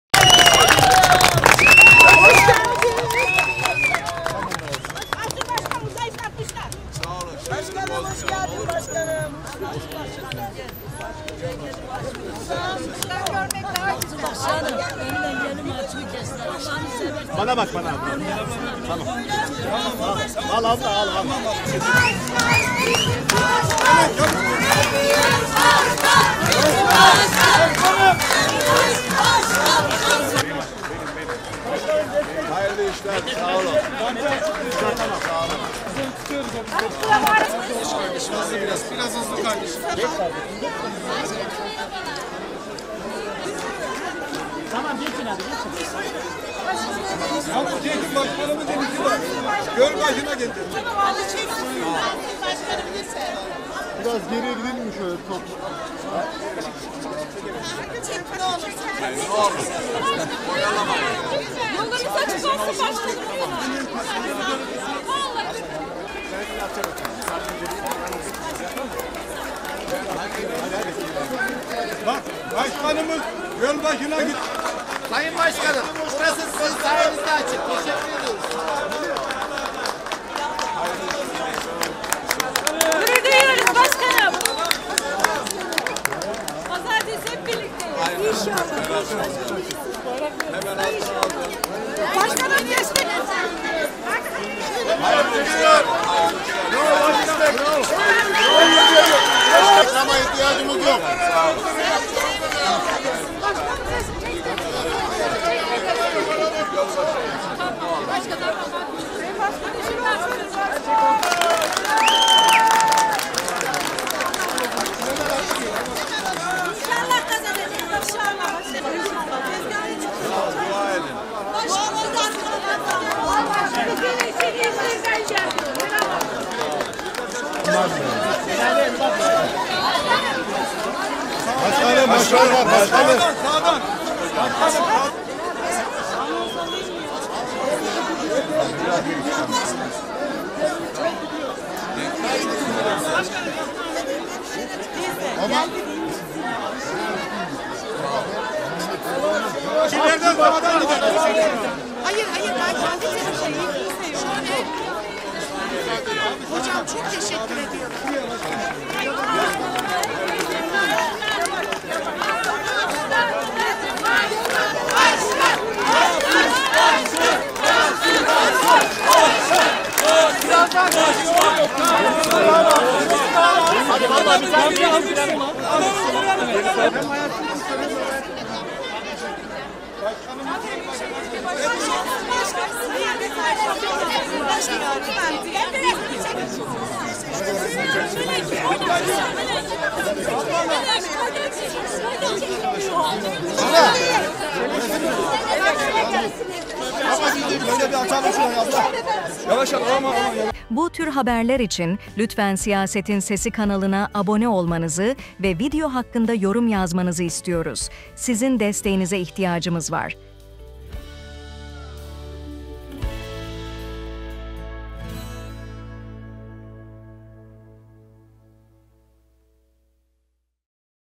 Mansur Yavaş Pazarı Ziyaret Ettiğine Pişman Oldu! Yoğun İlgiden Yürüyemedi!
Ankara Büyükşehir Belediye (ABB) Başkanı Mansur Yavaş, Pazar Yerleri Esnaf Odası Başkanı Recep Ayhan ile birlikte Sıhhiye'de bulunan Yenişehir Pazarı’nı ziyaret etti.
ABB Başkanı ve CHP Başkan Adayı Mansur Yavaş, bugün Pazar Yerleri Esnaf Odası Başkanı Recep Ayhan ile birlikte Sıhhiye'de bulunan Yenişehir Pazarı’nı ziyaret etti. Vatandaşların yoğun ilgisi nedeniyle yürümekte zorlanan Yavaş, esnafa ‘hayırlı işler’ dileğinde bulunurken; vatandaşların da fotoğraf çektirme taleplerini geri çevirmedi.